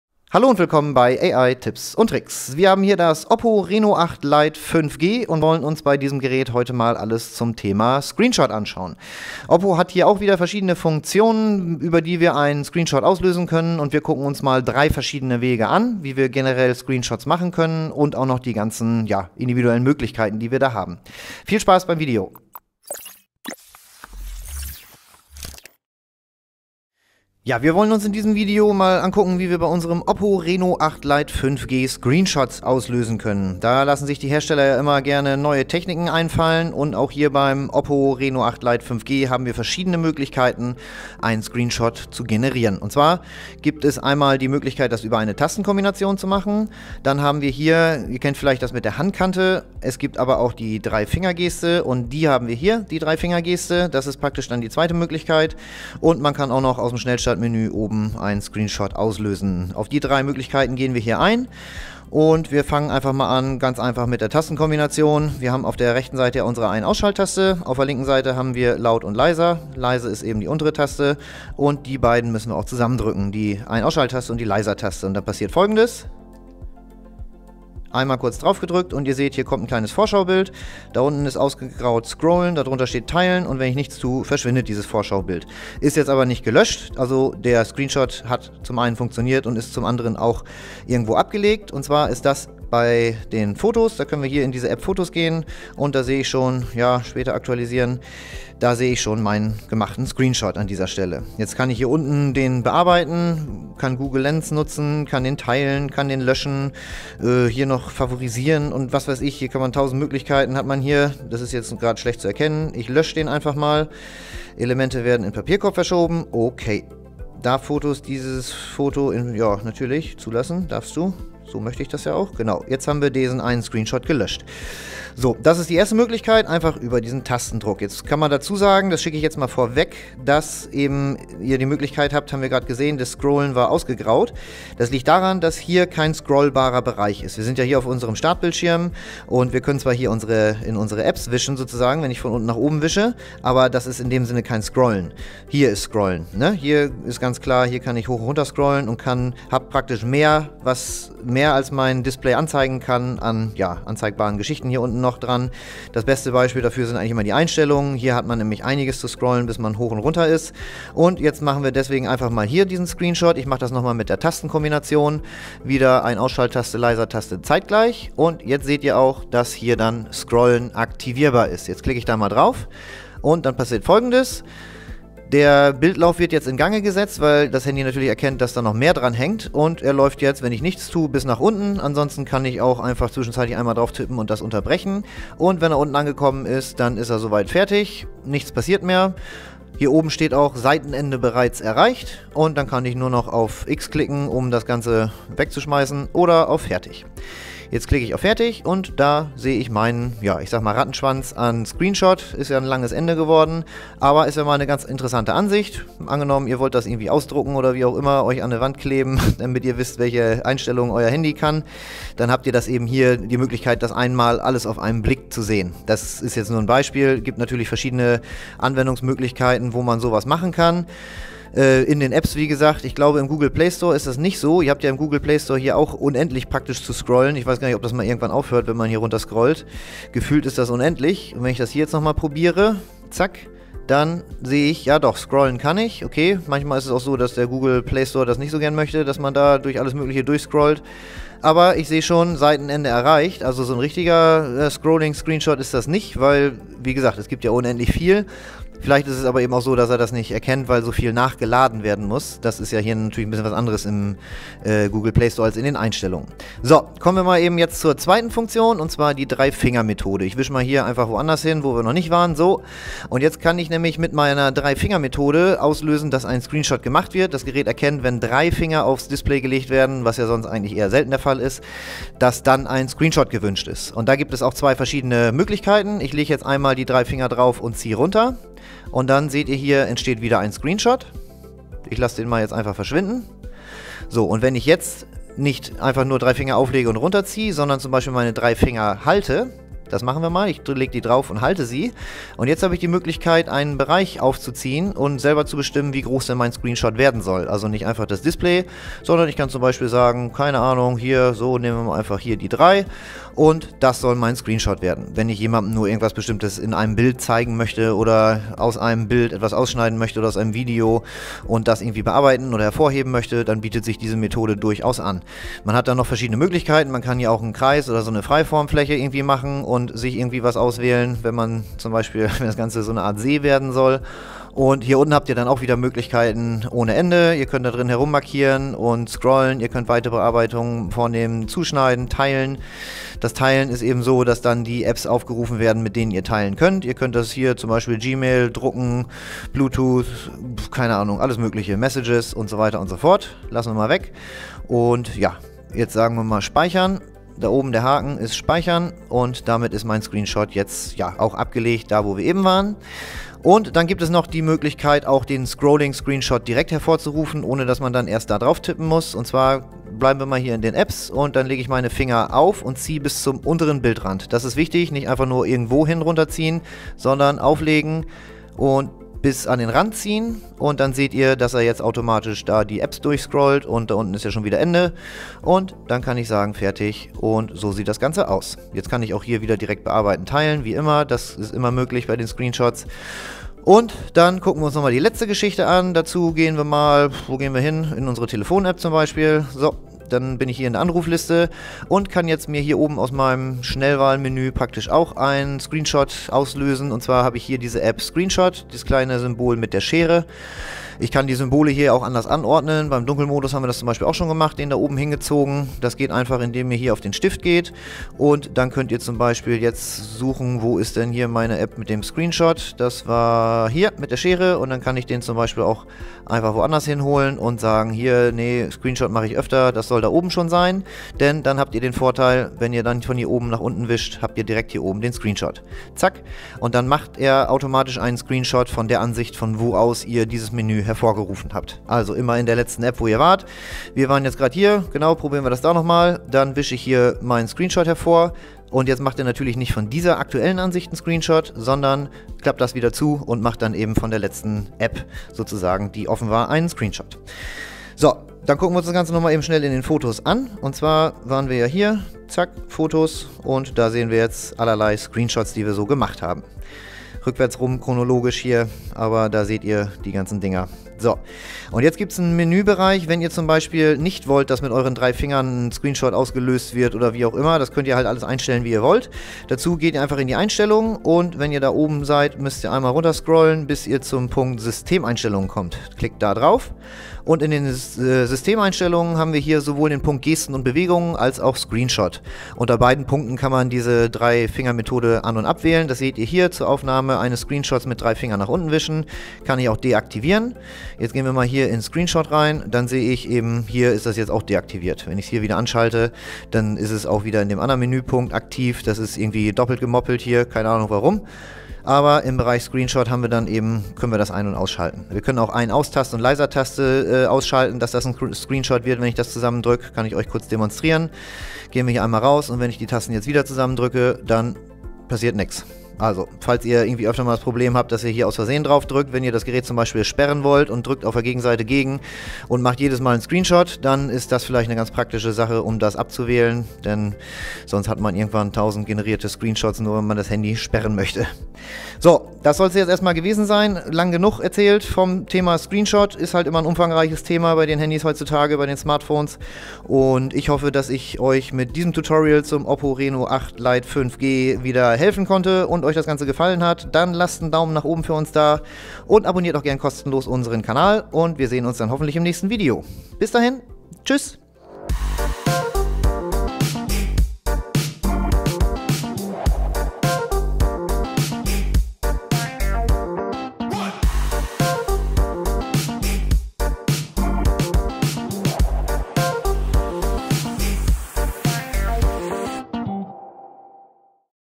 OPPO Reno8 Lite 5G - Screenshot erstellen • 📱 • 📸 • 🏞 • Anleitung | Tutorial
In diesem Video dreht sich alles um das Thema Screenshot erstellen beim OPPO Reno8 Lite 5G und wir zeigen Euch hier, wie es geht. Beim Reno8 Lite können wir über eine einfache Tastenkombination einen Screenshot erstellen, sowie durch das Auflegen von drei Fingern oder aber auch aus der Übersicht aller aktiven Apps heraus. Mit dem erstellten Screenshot können wir natürlich auch einiges anstellen und obendrein gibt es ja auch noch die Funktion des Scrolling Screenshot. Wir zeigen Euch hier alles um das Thema Bildschirmfoto beim Reno8 Lite 5G von Oppo. Viel Spaß dabei.